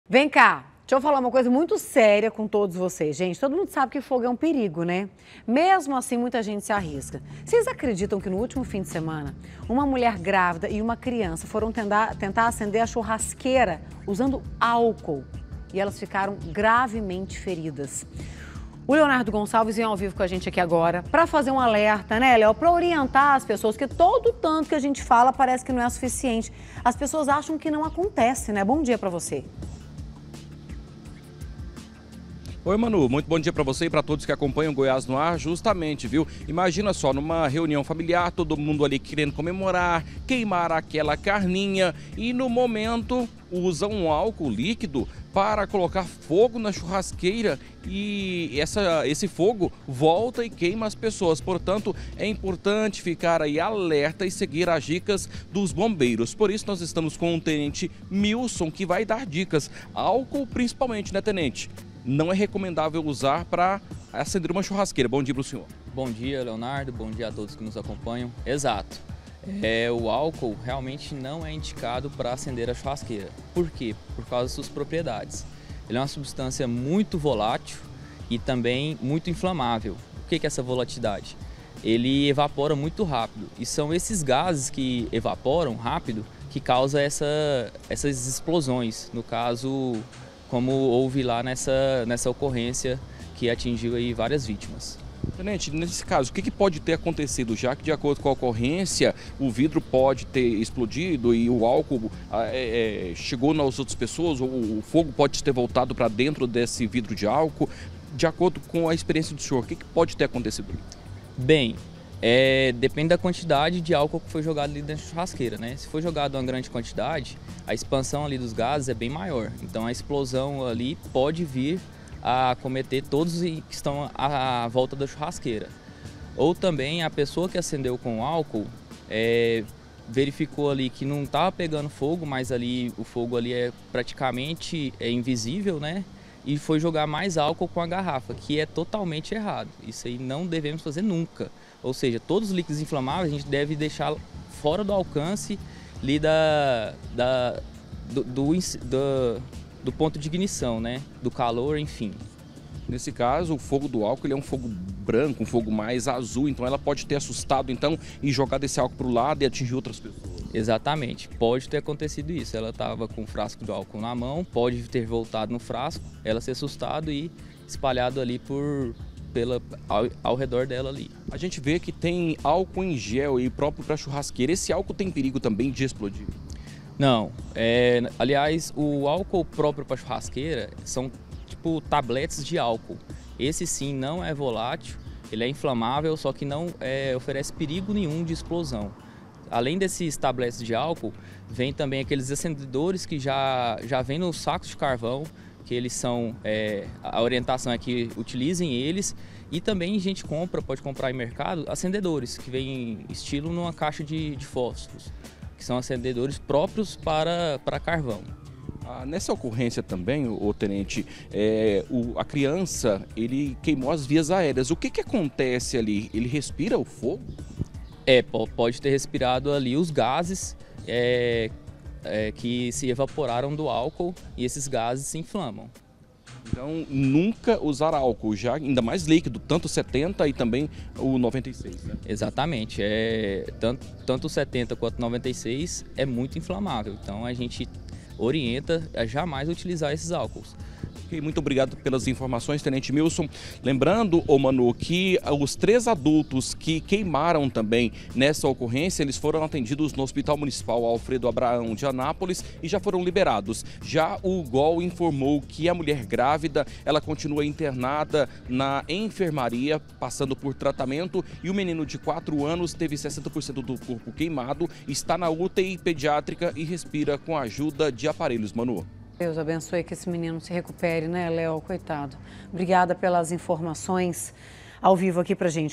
CUIDADO COM O FOGO: GRÁVIDA E CRIANÇA SE FERE GRAVEMENTE
TODO MUNDO SABE QUE FOGO É UM PERIGO. MESMO ASSIM MUITA GENTE SE ARRISCA. NO FIM DE SEMANA UMA MULHER GRAVIDA E UMA CRIANÇA DE CINCO ANOS FICARAM GRAVEMENTE FERIDAS AO ACENDEREM UMA CHURRASQUEIRA COM FOGO.